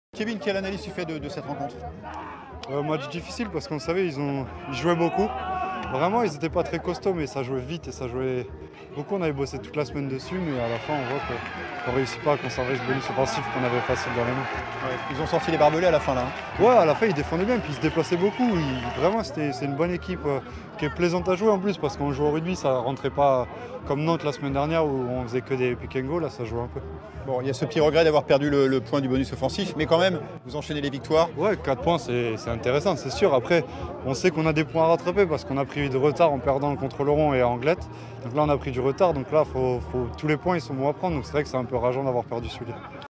Rouen s'impose face à Saint-Jean de Luz
4e victoire consécutive ce dimanche (30 octobre 2016) du Stade Rouennais